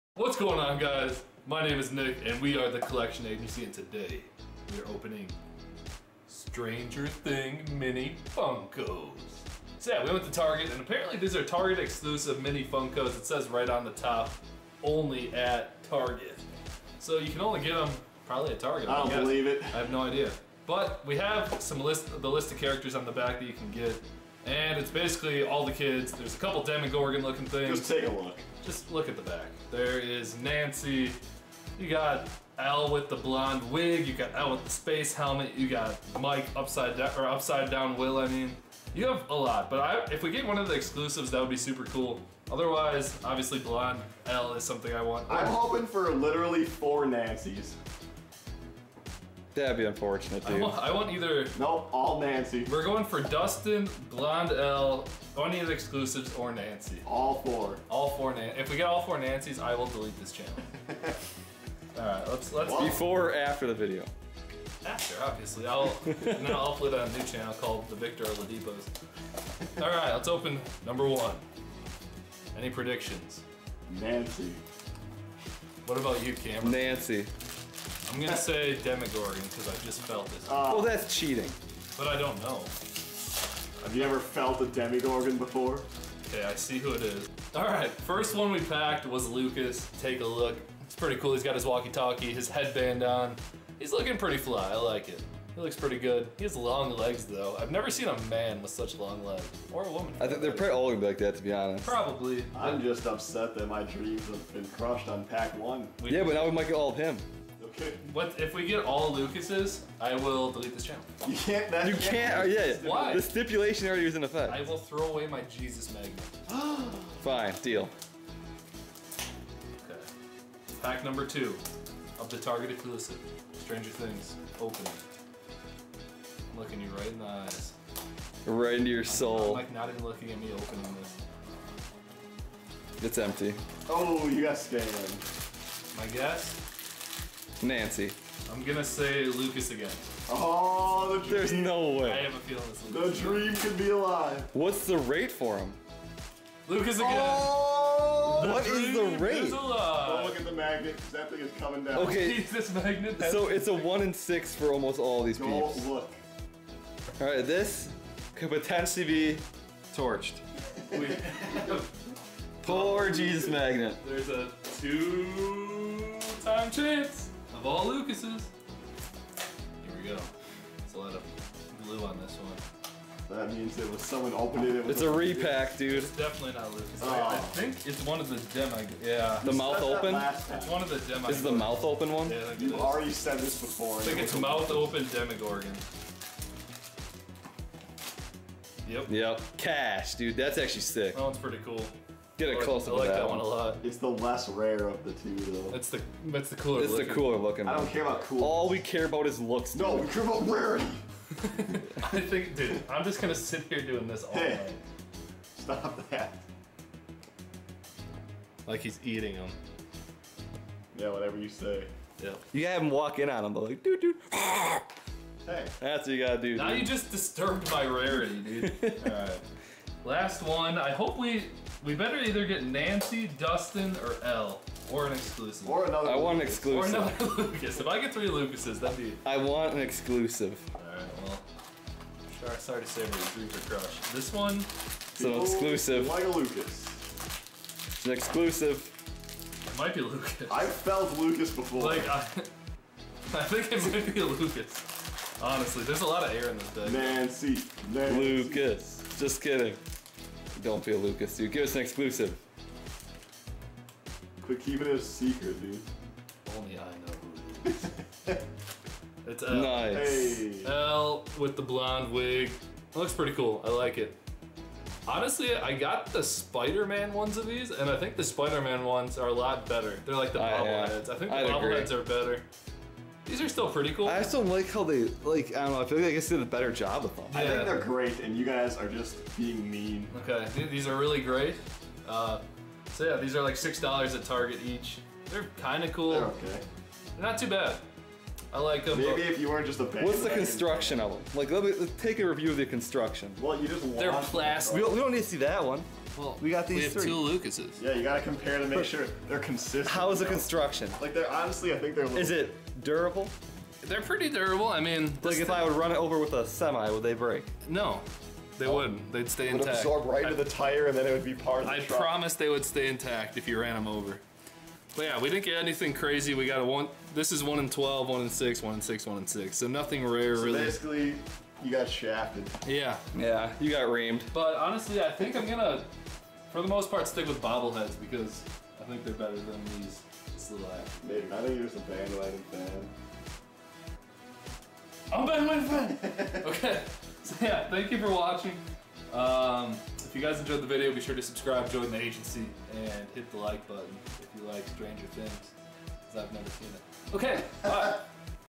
Stranger Things | Funko Mystery Mini Opening! (Stranger Things Funko Pop Target Exclusive)
Season 3 of Stranger Things is out and we have some Stranger Things Mini Mystery Funko Pops ready to open!!! Eleven or El, Dustin, Will, Lucas, Mike, and even a demogorgon make an appearance in this series of Funko Pops! These are a Target exclusive.

Stranger Things | Funko Mystery Mini Opening!